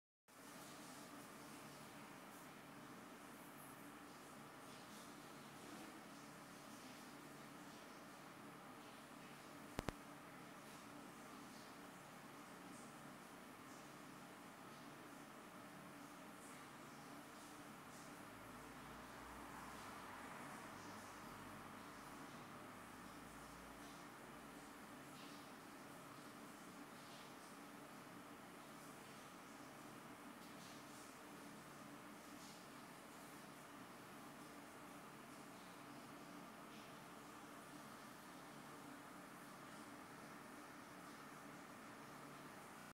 PDP AFTERGLOW 手把LED色彩循環模式展示
此為將發光模式切換成色彩循環模式後的LED顏色變化。手機錄影雜訊有點多，請多見諒。
開箱文連結
測試環境  windows 7 ，windows 7 需要下載官網驅動，驅動連結開箱文有。